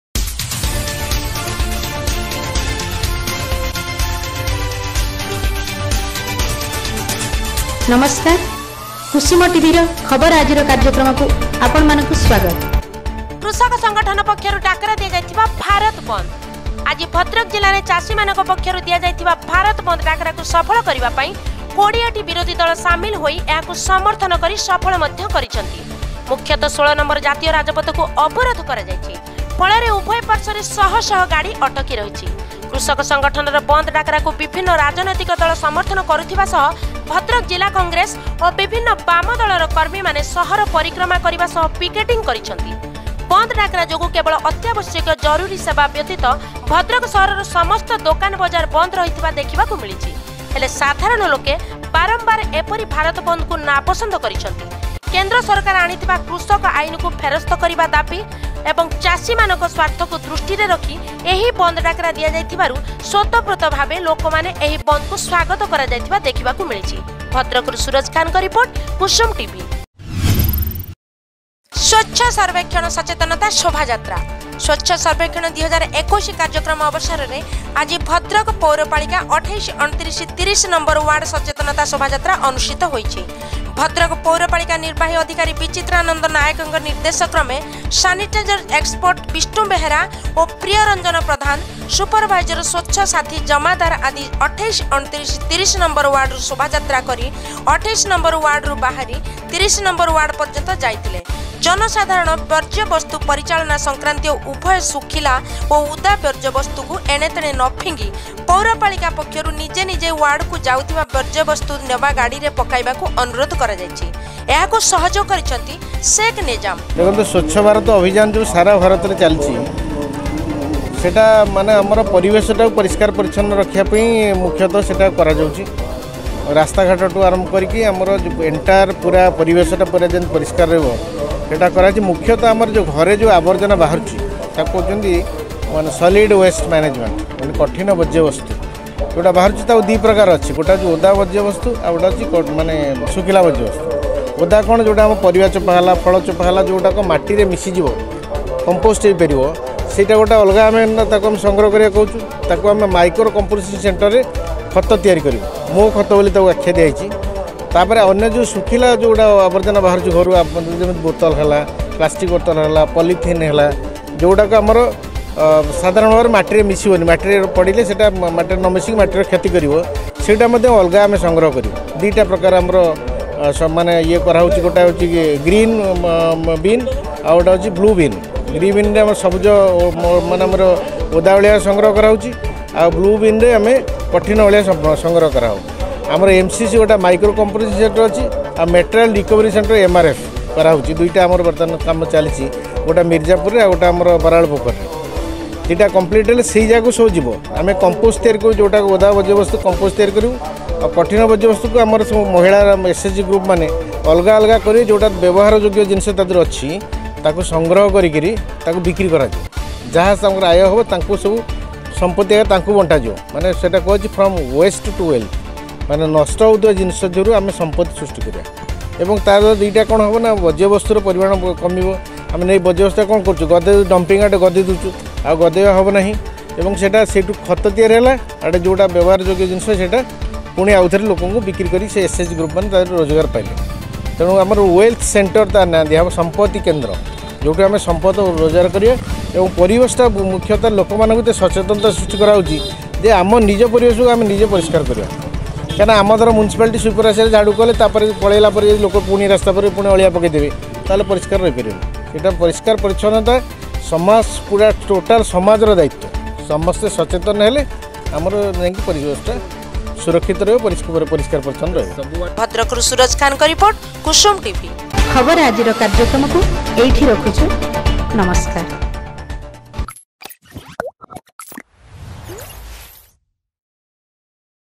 Khabar Aajira -09 12 2020Morning News
Bharat bandh#
swachha bharat sarbekhyana 2021 ra sachetanata rally#
Bhadrak ru Suraj Khan